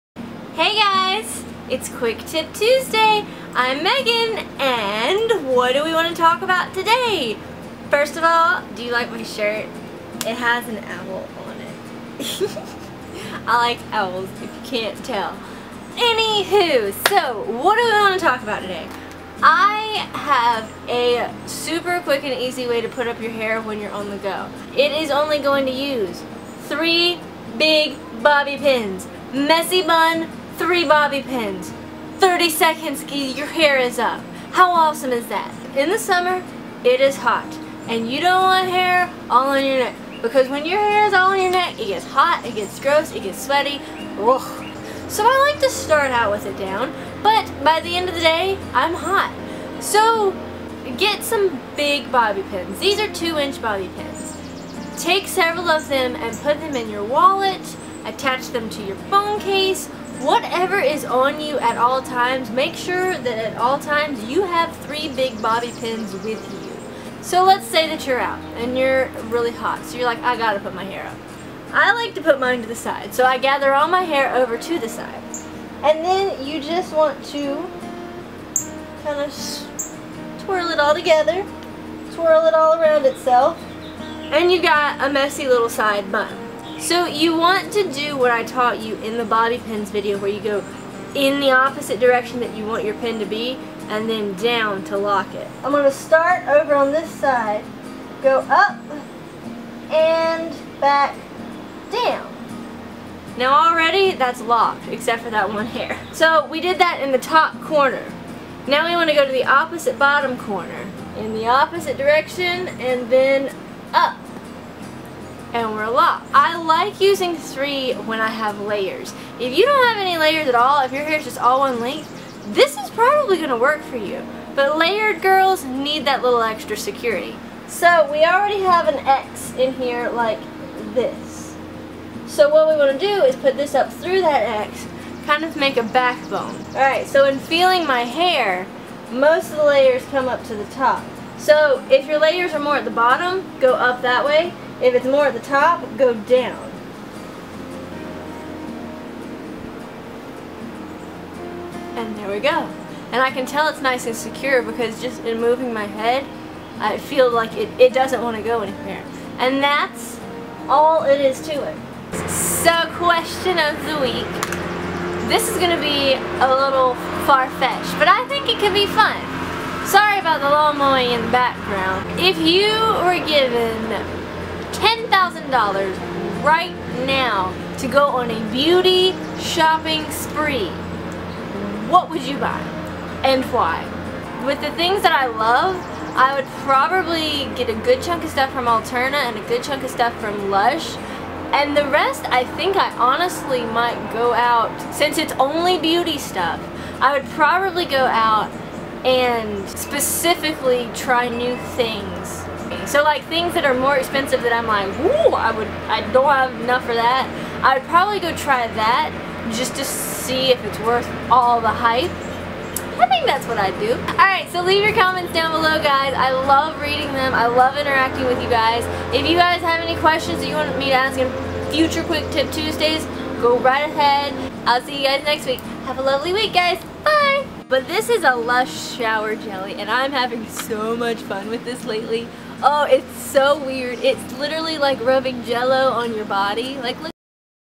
Quick Tip Tuesday: Messy Bun in Less Than One Minute Using Only THREE Bobby Pins
Get out the door fast with this super fast way to pin a cute messy bun!  The best part is, as long as your have 3 bobby pins, this can be done ANYWHERE!  Open for more!

Hey lovely ladies (and gents if your watching!) !  This weeks Quick Tip Tuesday dives into something great for summer, literally taking 3 big bobby pins, making them an X and then giving them a backbone and getting a fun messy bun that took you less than a minute!  How awesome is that?  :] I honestly do this all the time when I'm out, because I get hot and don't want my hair down anymore.

Stay lovely!